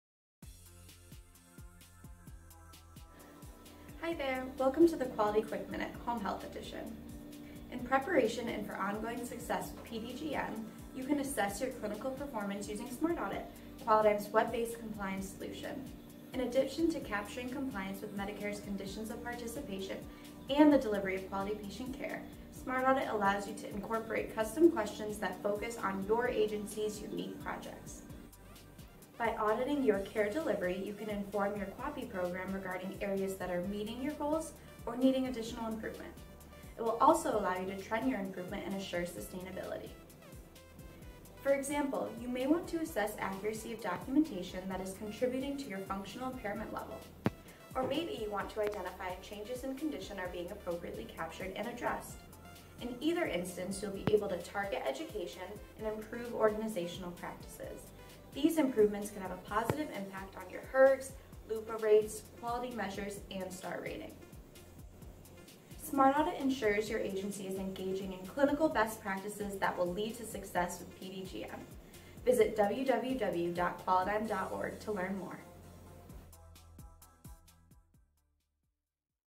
Quality Quick Minute - Make a Smooth Transition to PDGM with SmartAudit
In this edition of the Quality Quick Minute, Marissa Moncata discusses how your agency can utilize SmartAudit, Qualidigm's web-based compliance tool, to inform your PDGM efforts!